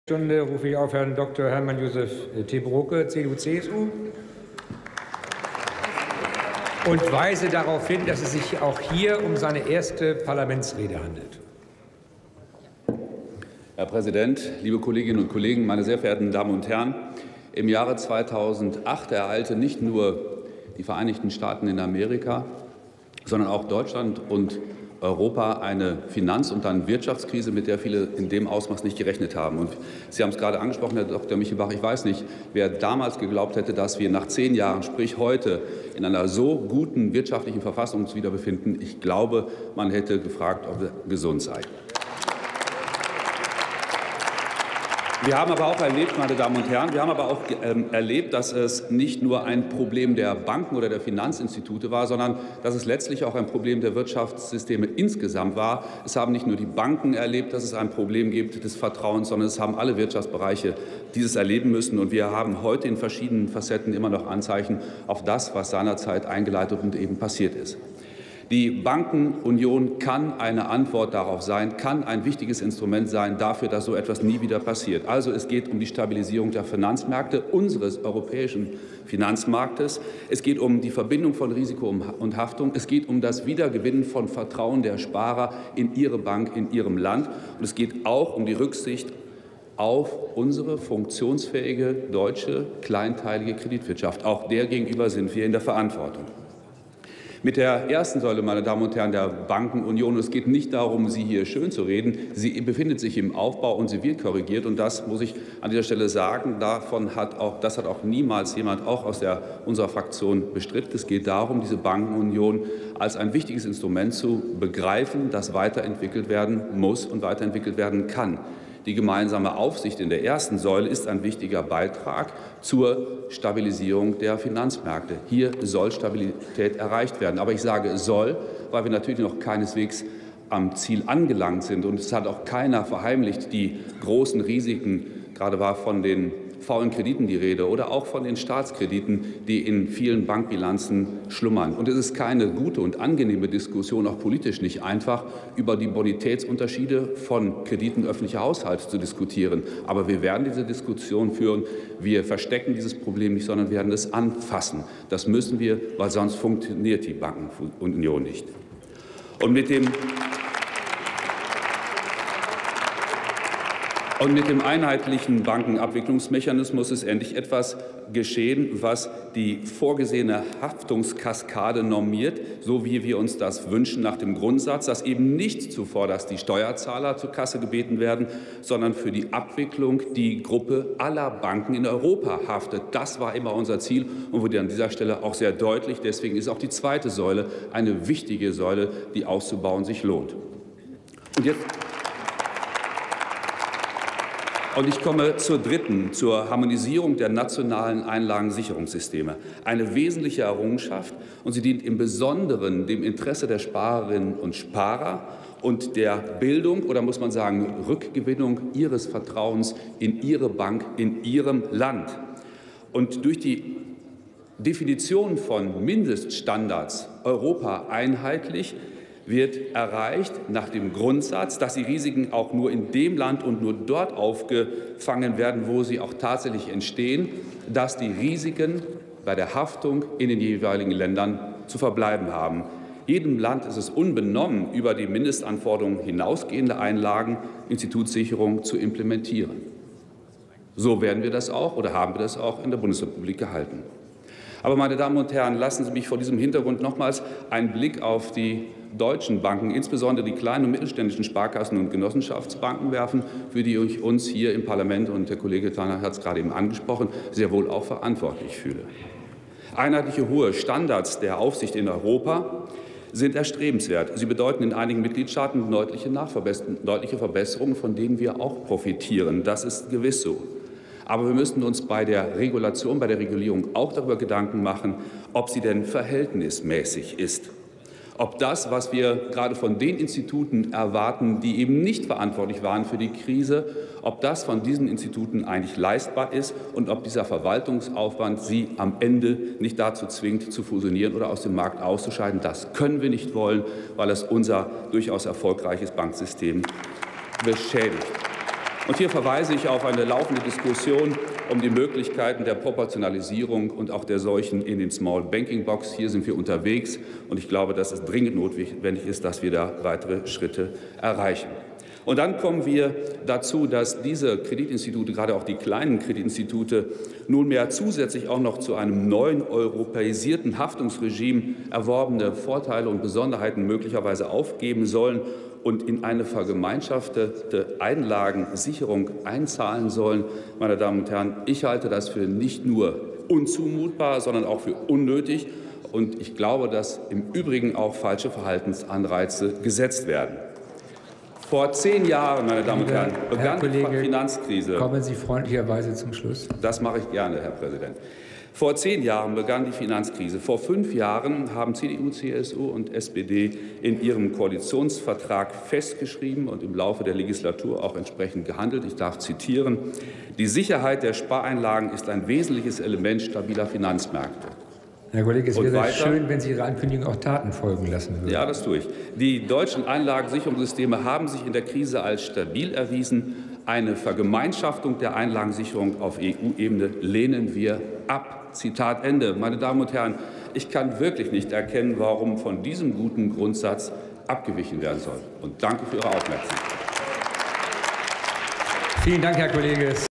Hermann-Josef Tebroke: Aktuelle Stunde zu einer europäischen Bankenunion [Bundestag 31.01.2018]
Aktuelle Stunde
auf Verlangen der Fraktion der FDP
Positionierung der Bundesregierung zu einer europäischen Bankenunion

18. Deutscher Bundestag
10. Plenarsitzung
Tagesordnungspunkt: Aktuelle Stunde zu einer europäischen Bankenunion
Redner: Tebroke, Dr. Hermann-Josef, CDU/CSU
Partei: Christlich Demokratische Union (CDU)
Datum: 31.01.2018
Plenarprotokoll:
UVID:7197620